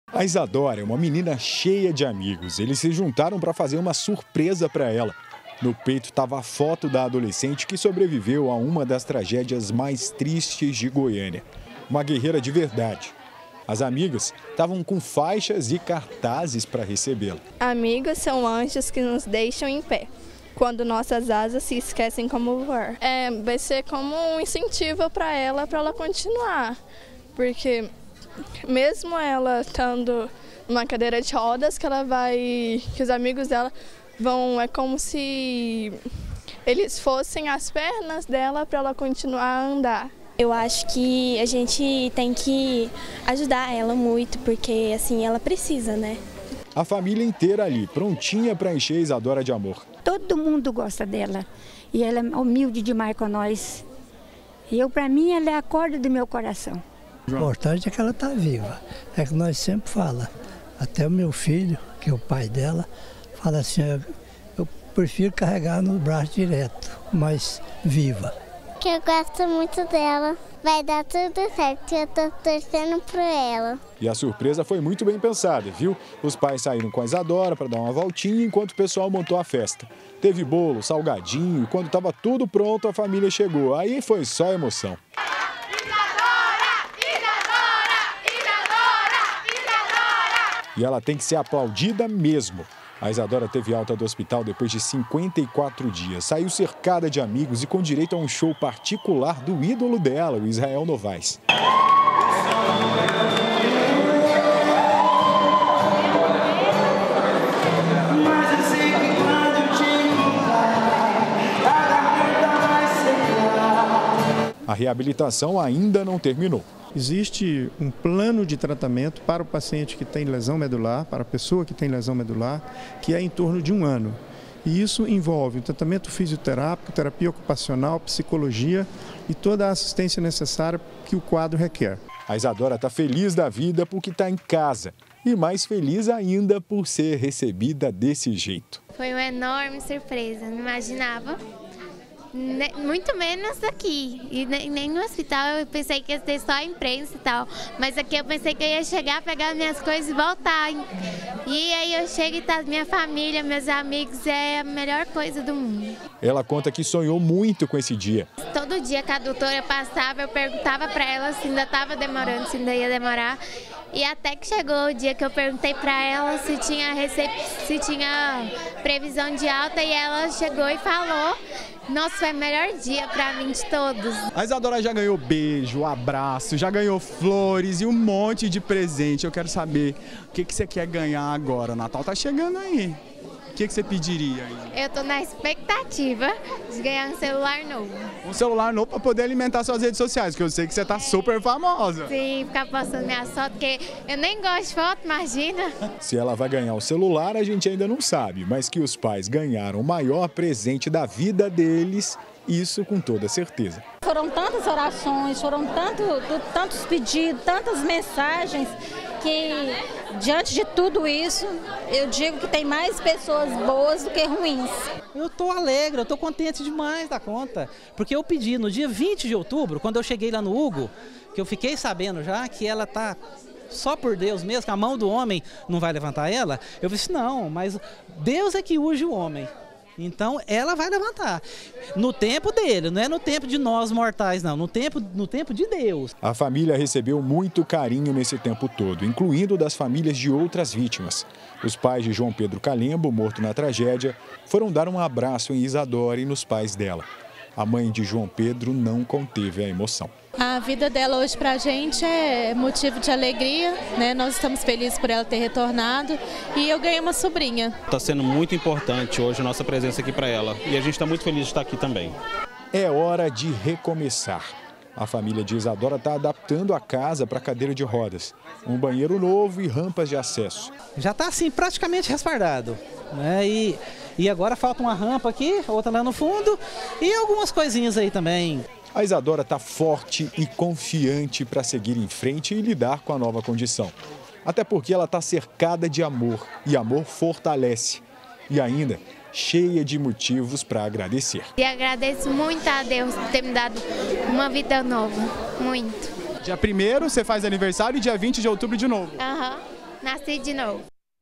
GA - Isadora recebe homenagem de amigos - 14-12-2017
Depois de cinquenta e quatro dias no hospital, Isadora de Morais, uma das sobreviventes do atentado ao Colégio Goyases, voltou pra casa. E na chegada, foi recebida por parentes e amigos, numa festa linda.